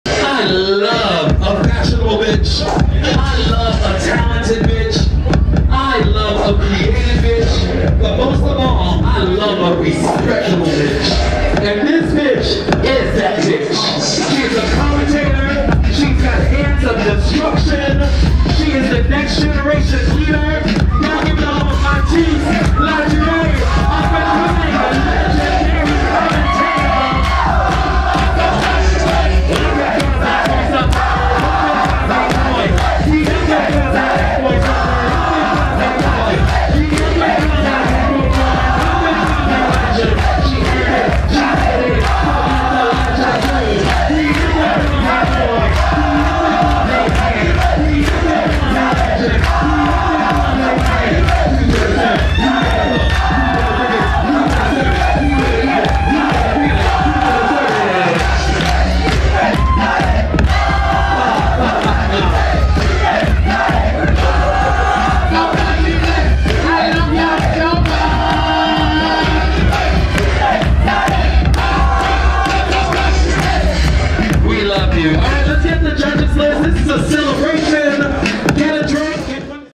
The International Mother MC MATYOUZ LADUREE at The Stripes AfterMath Ball
The LSS of The International Mother MC MATYOUZ LADUREE at The Stripes AfterMath Ball, by Mr.Alvaroo & Habibitch Gorgeous Gucci, Matyouz LaDurée & Gigi Revlon at The Dock B (Paris).

  To celebrate the birthday of our very dear Mother Nikki, nothing better than offering her a ball 🥳!
So join us in celebrating Mother Gorgeous Nikki Gucci’s 25 years of Ballroom career, the one who paved the way we are walking in France and Europe, as well as her Up and Coming Icon status 🤩